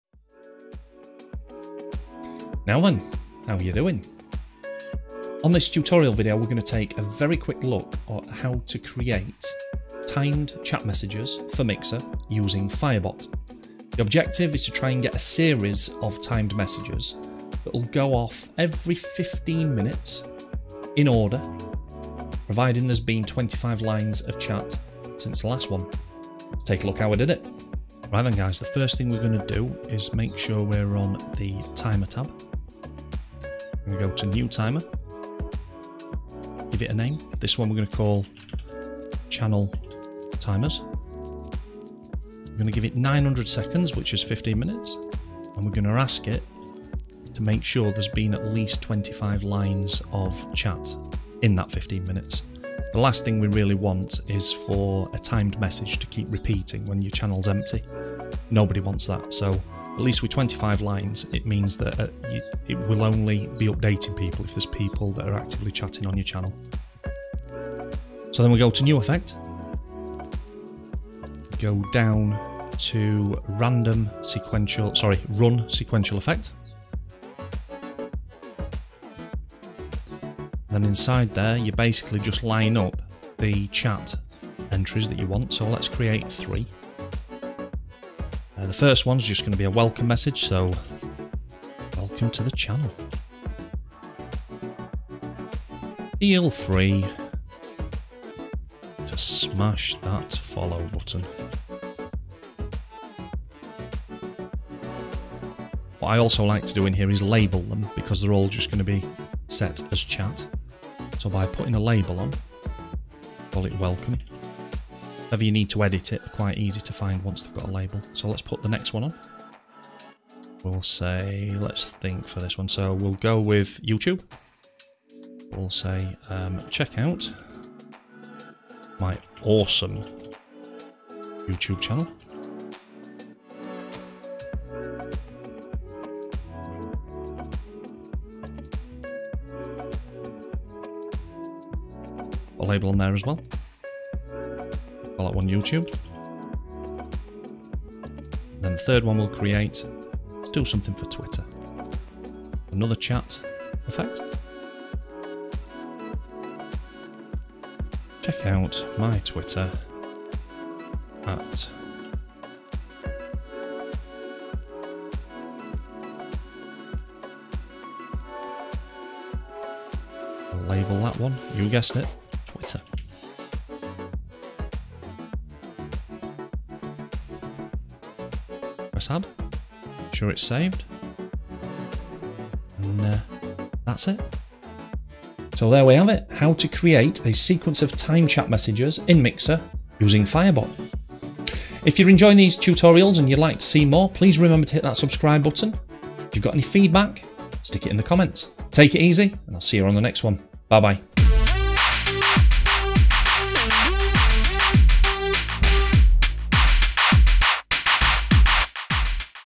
How to Setup Firebot Timers
It took me a while to work out how to setup Firebot Timers so wanted to share my findings here.  Timers allow you to put chat messages into your Mixer channel whilst you're broadcasting to remind people about following on social networks etc.